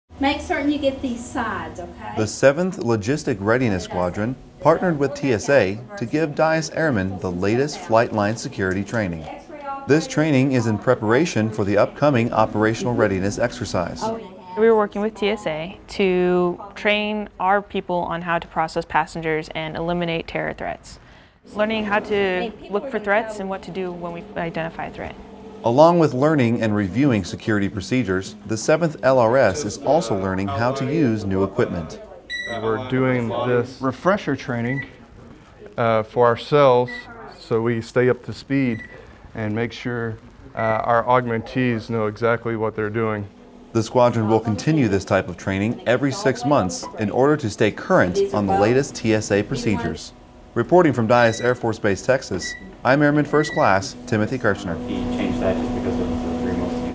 TSA Training
The 7th Logistic Readiness Squadron received flight line security training from TSA.